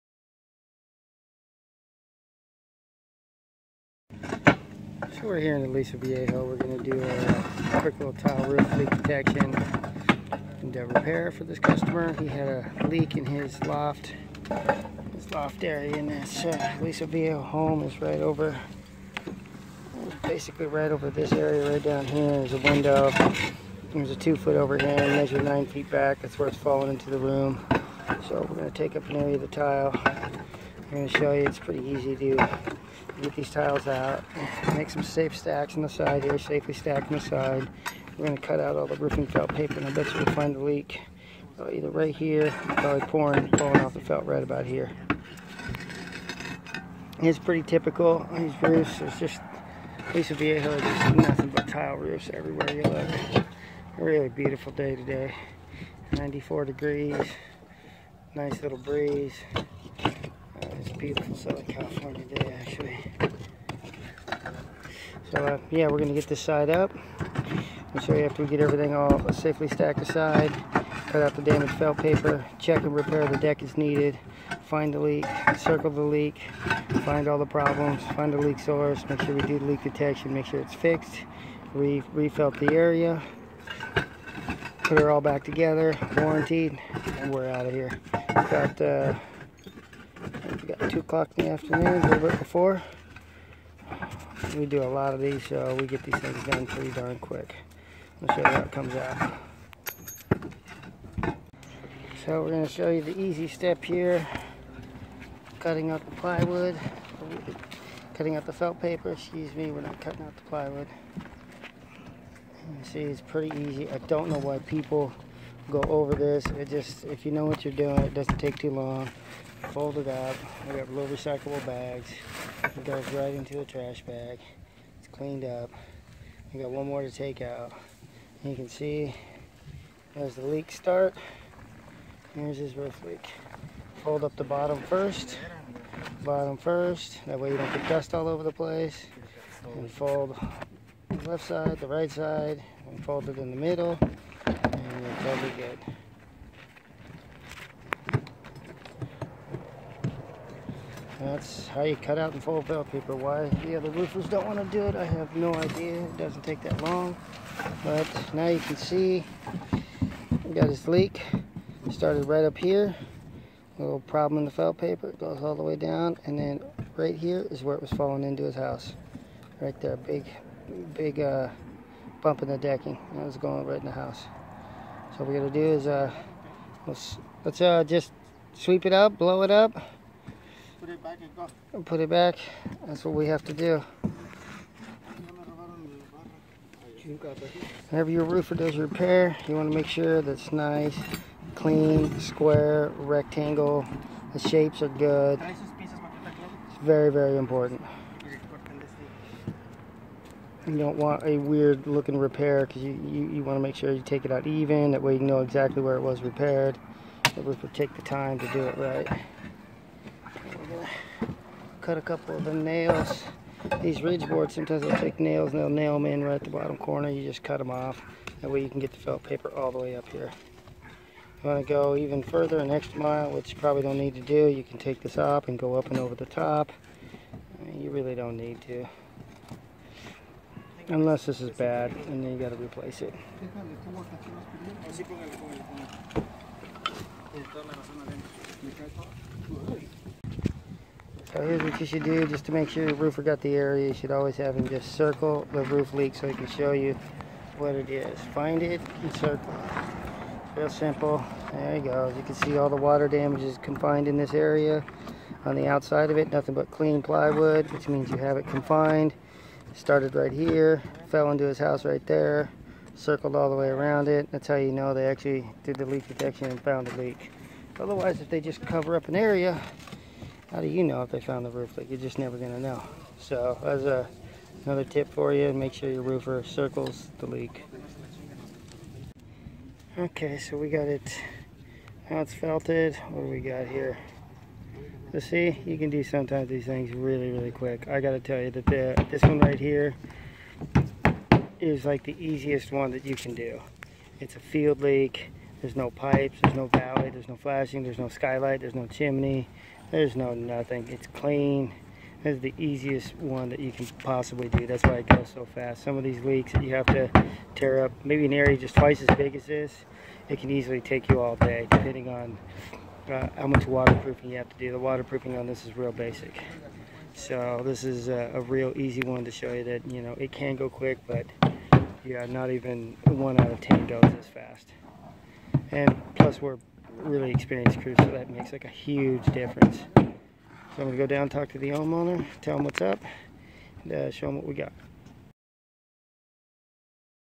Fastest tile roof leak repair from start to finish...In and Out in under an Hour.
The one main detail you need to have done when hiring a roofer for finding and repairing a leaking roof is the removal of the felt paper down to the deck.  All of the roofing needs to be removed around the roof leak area in order to inspect the roof for mold damage under the roofing tiles.

In this short video we show how quickly this can be done for a small area and a simple roof design.  The type of roofing tile on this repair is concrete tile in the "M" tile profile.  These tiles are sometimes referred as "low rise" and are known to have a Mediterranean or Villa style or look.

This roofing video shows how to properly repair a leaking tile roof from start to finish.  DIY roof repair tips and tricks.

More about this repair:

This is a tile roof leak repair we started in the afternoon.  I went in to see what the problem was and the customer hired us on the spot.  The leak happen to be a concrete tile roof and the leak was in a field area.  I knew it would be a quick.  This video shows how quickly you can remove the tiles, cut out the damaged felt paper, find and circle the roof leak, apply new felt paper, seal it up, and install the tile back together "if" you are properly trained.